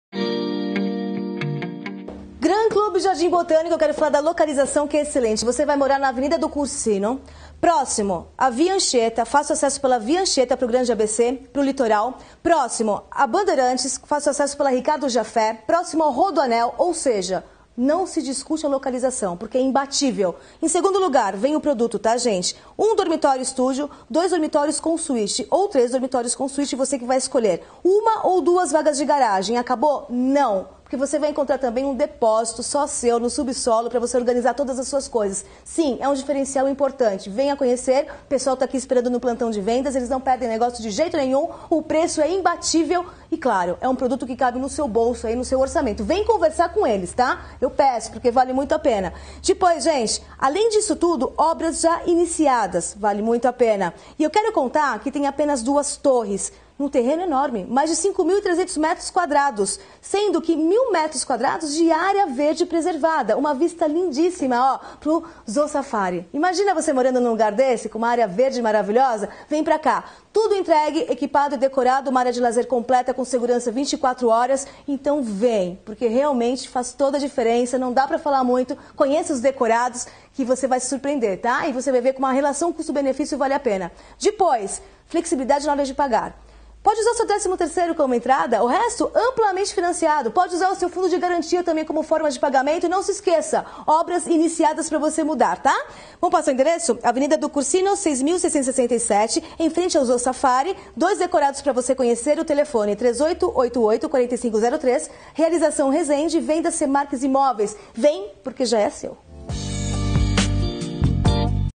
C MARQX - Grand Club Jd. Botânico
1 Dormitório (studio), 2 e 3 Dormitórios (com suíte).
Sala para 2 ambientes, banheiro social, cozinha e área de serviço - Depósito privativo e Vagas no subsolo. Áreas privativas de 35,79m² à 72,76m².

Jd. Botânico / Saúde

Um verdadeiro complexo de bem-estar.

Uma oportunidade única e inédita de morar na cidade, com qualidade, bem-estar e conforto. Sintonizado aos mais modernos conceitos de qualidade de vida, o projeto do Grand Club Jardim Botânico tem como diferencial o destaque da implantação em meio a um verdadeiro oásis urbano.

Seus apartamentos se inserem em um verdadeiro clube com lazer e relaxamento, que terá os mais modernos equipamentos e conceitos de lazer, convívio coletivo, introspecção e cuidados com o corpo e a mente.